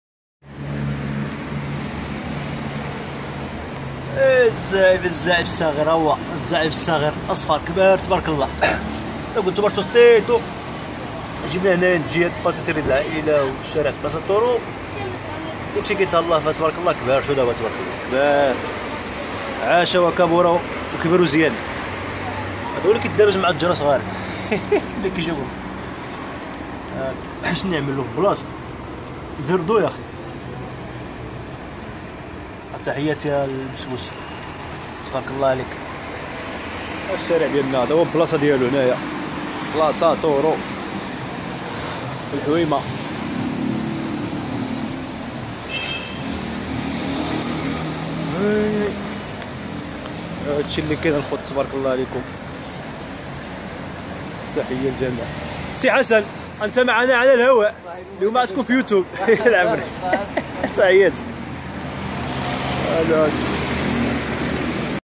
القط الأصفر الصغير المشاكس للكلاب يكبر وفي صحة جيدة
مبادرة إطعام القطط الجائعة في الشارع زمن كورونا مستمرة وستستمر إن شاء الله تعالى. تحياتي للجميع 🐈 🐱 🐈